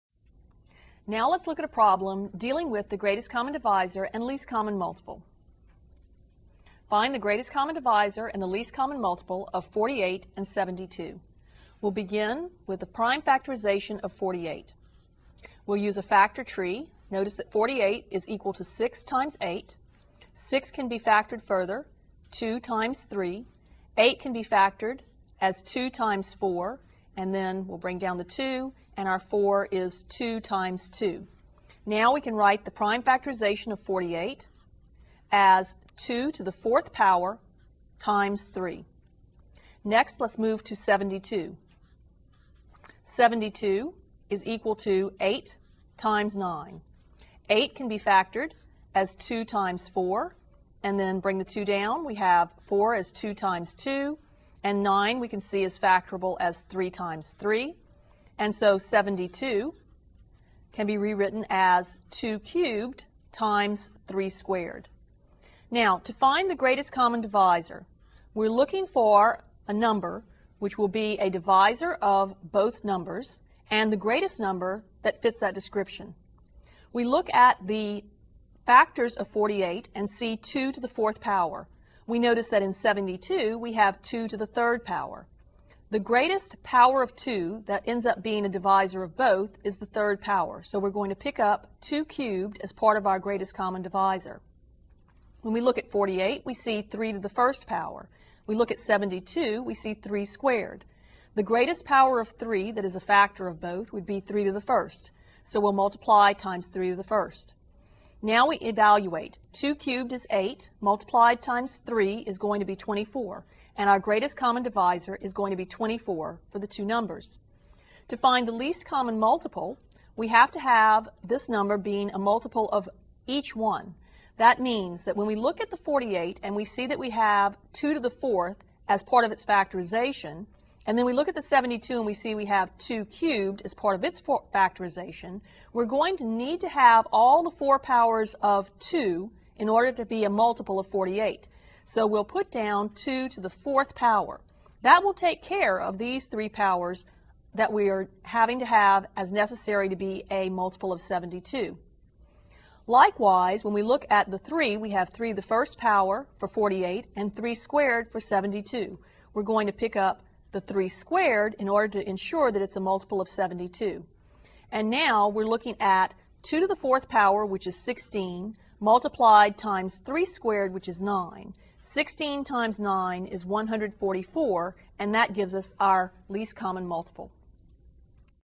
Blitzer Thinking Mathematically Ch 5 Ex 3
A professor shows how to find the greatest common divisor and least common multiple.